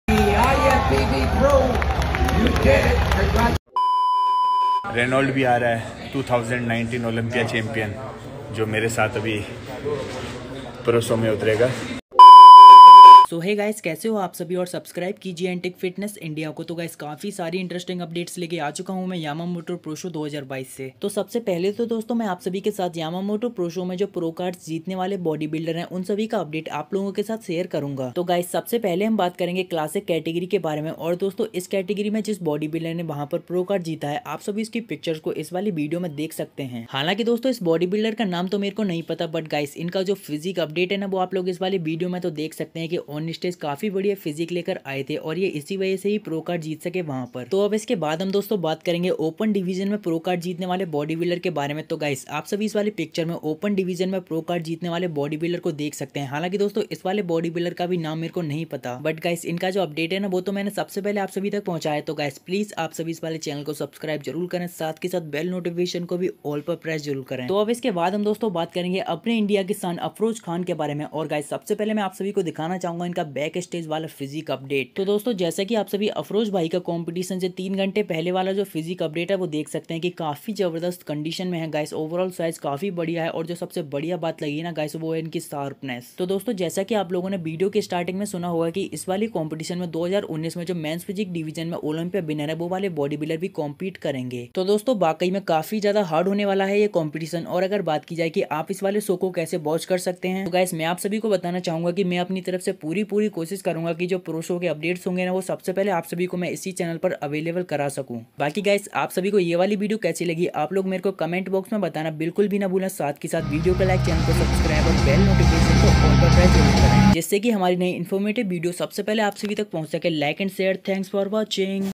Yamamoto 2022 Pro Card Winners + Afroz khan Last Update Yamamoto Pro Show 2022 + Bodybuilding Update
About this video - Yamamoto pro show pro card winners + Yamamoto pro show latest updates